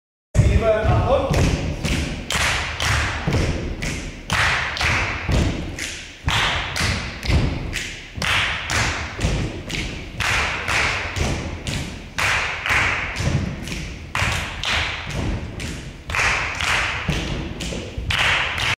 Grundtanz mit Klatschen und Schnipsen (6)
Die Stepper-Gymnastik wird  mit ergänzenden Lauten verknüpft. Diese Idee lässt sich für die Rhythmusschulung nutzen und auf verschiedene Schulformen übertragen.